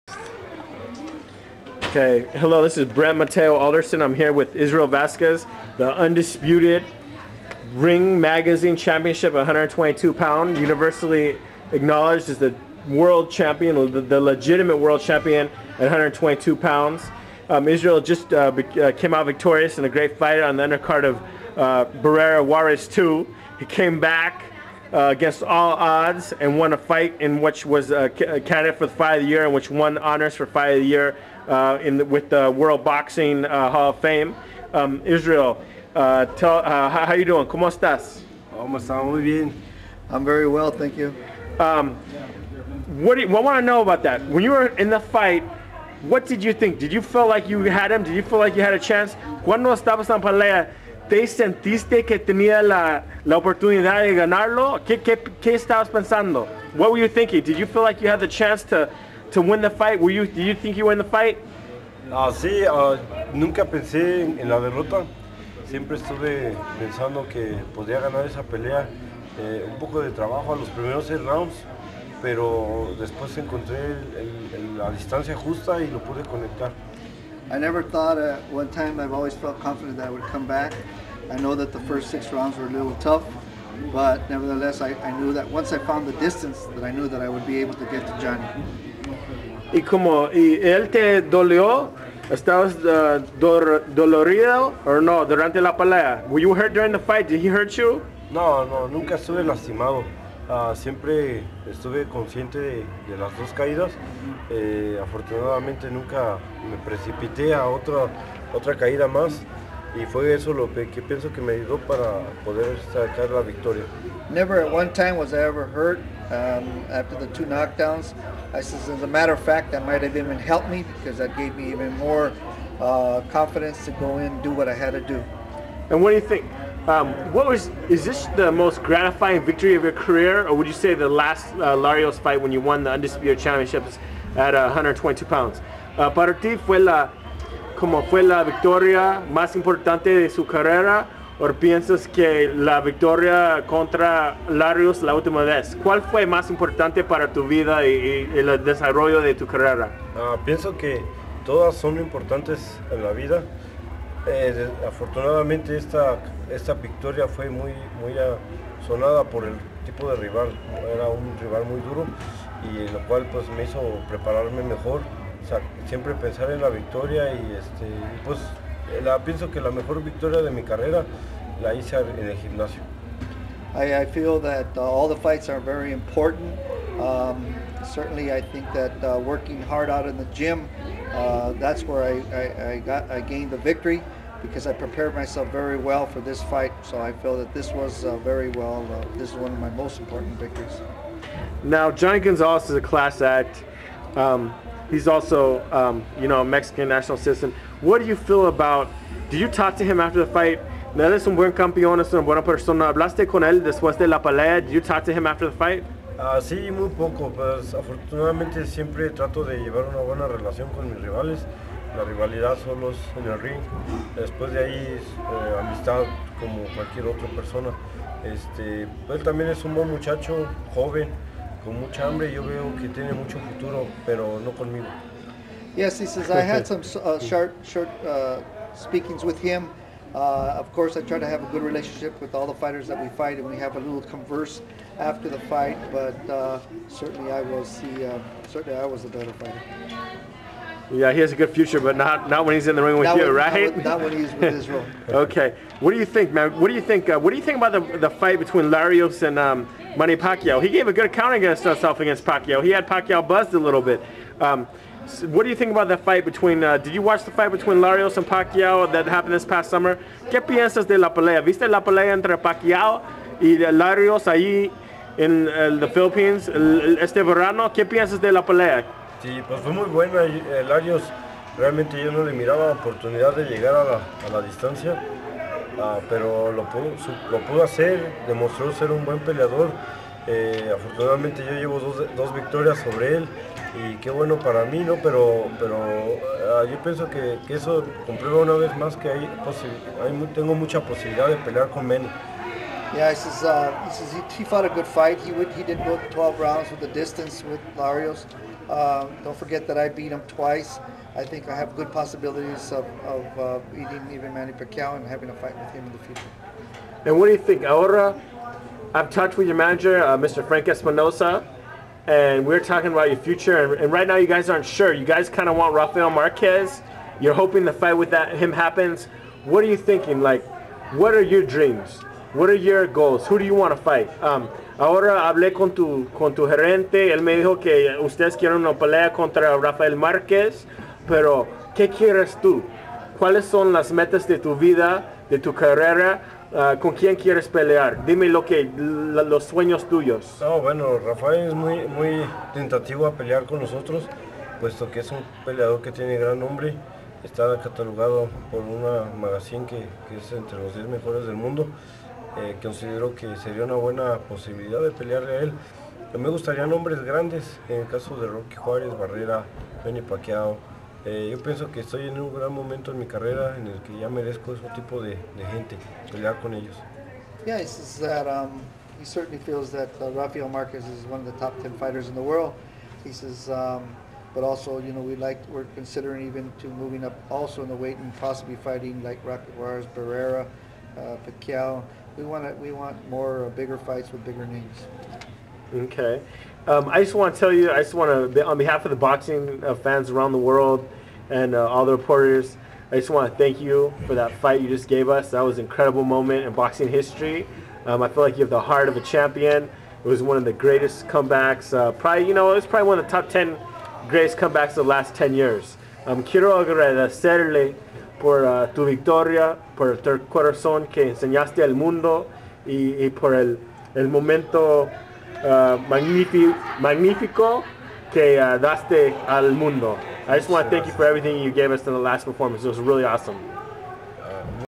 Israel Vasquez 9/2006 on fight with Gonzalez and a prospective bout with Marquez
Israel Vazquez talks about his come from behind victory over Johnny Gonzalez, his career, and a showdown with Rafael Marquez